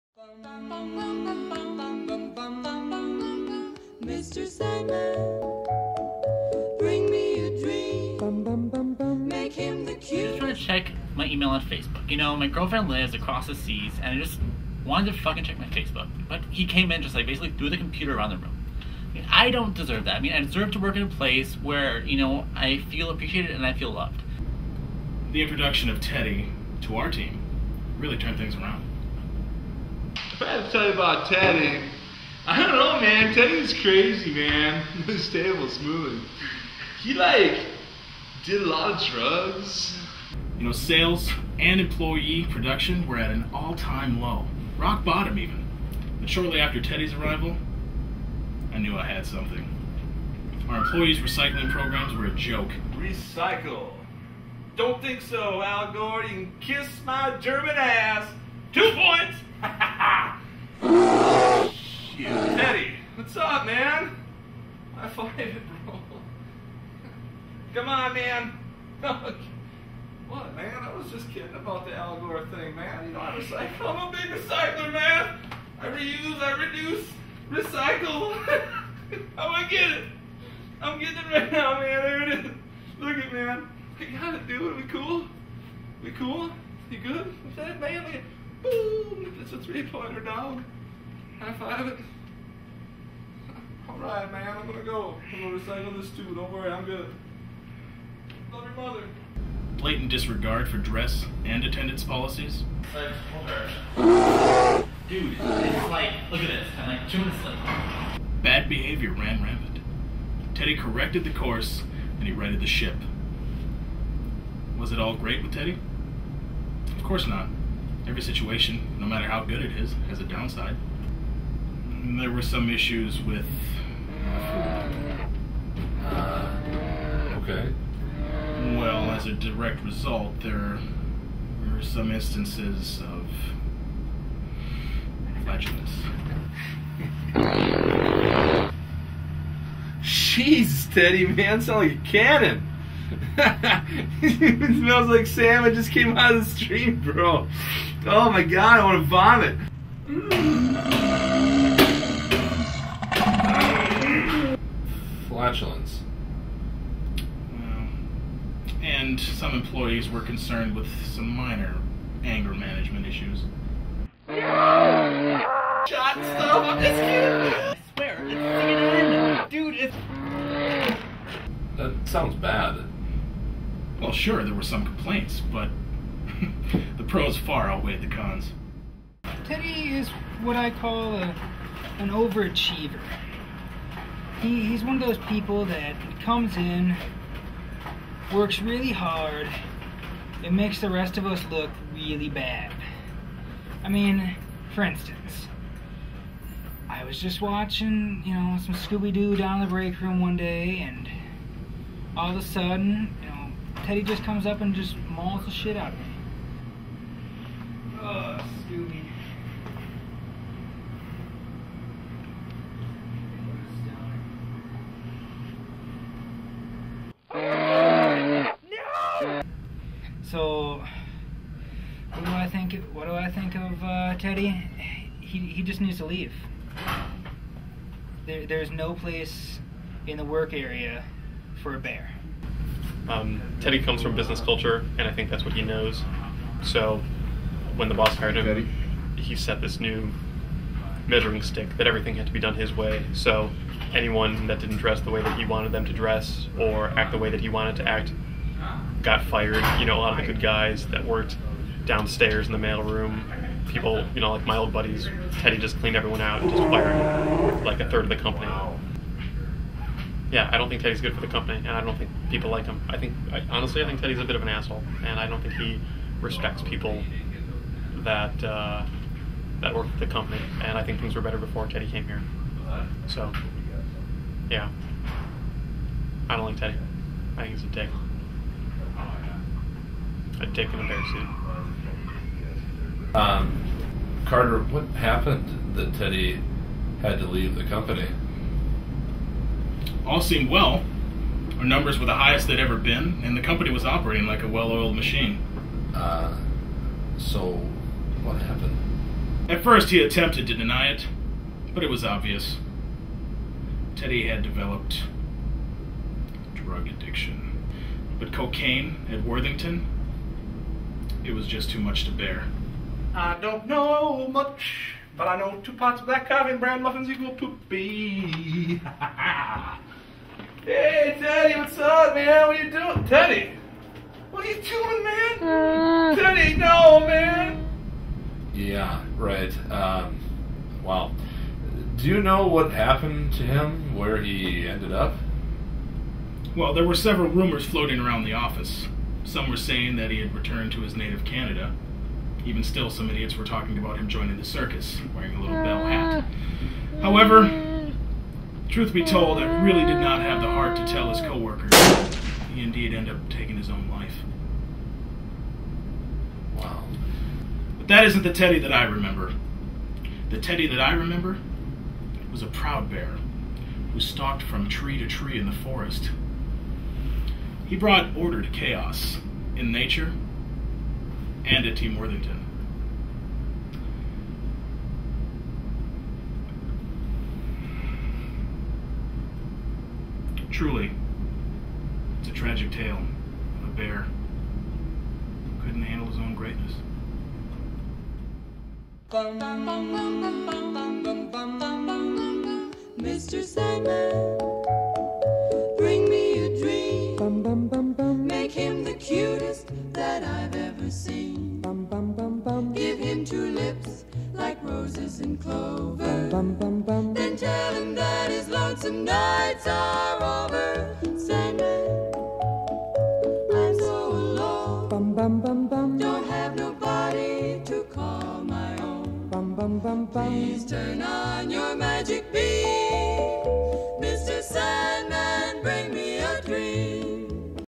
Teddy Bear
This movie is about Teddy, a bear who was hired to be an Office Manager. Teddy helps to increase the productivity of the company. This film shows the rise and fall of Teddy, as well as, how he has affected others.